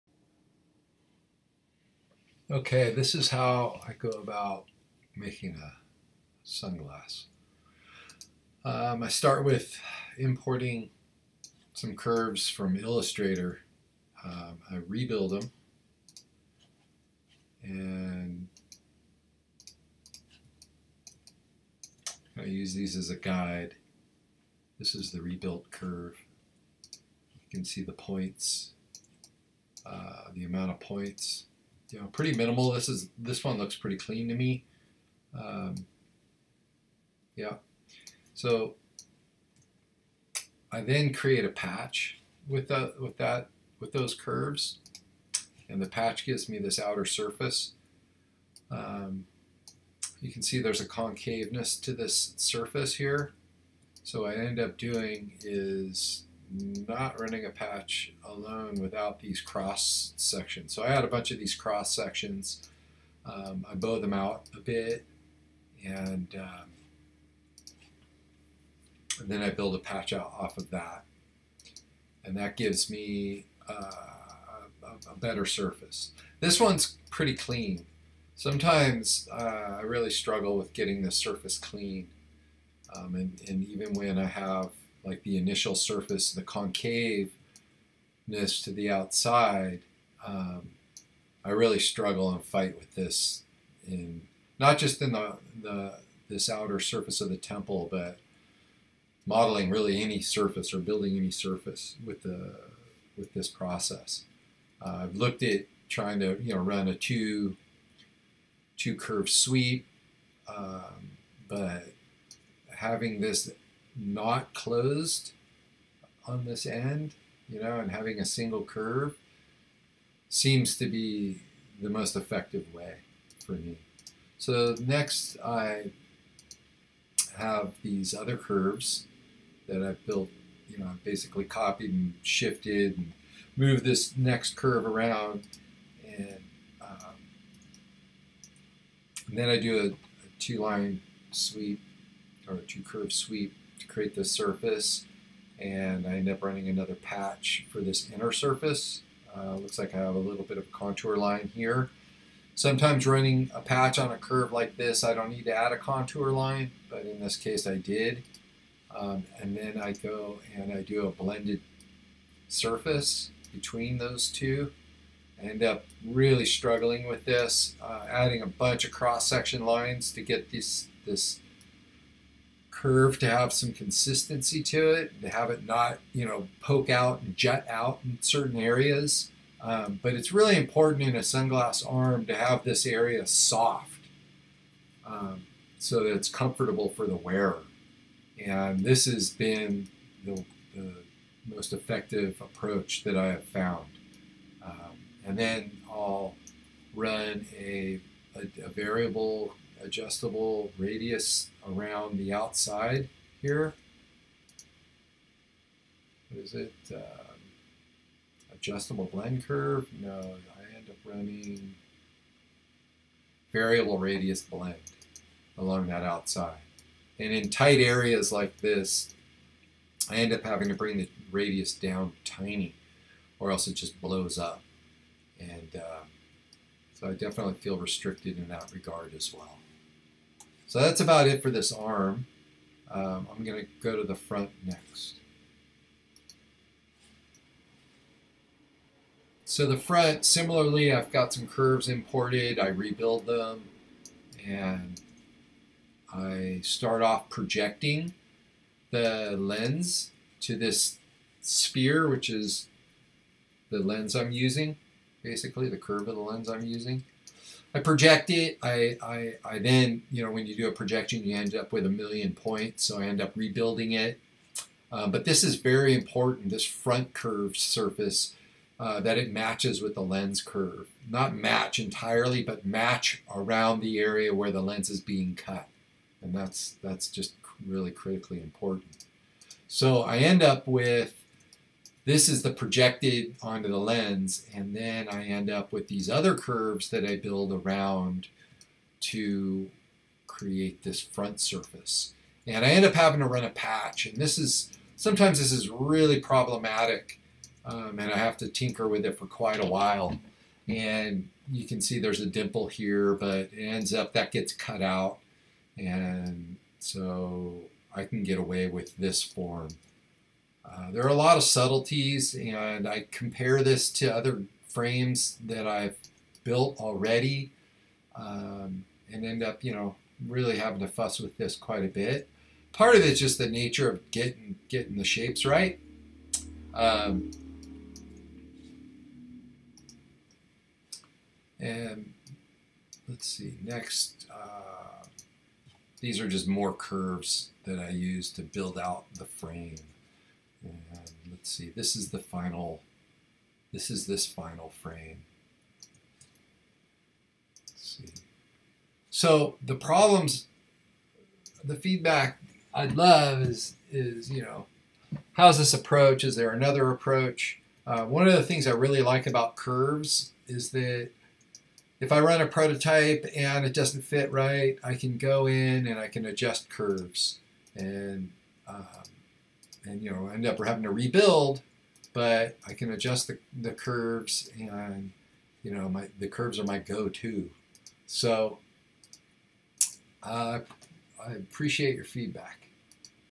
Rhino Sunglass Build 1
This is an intermediate level - quick overview of how I build eyeglasses in Rhino.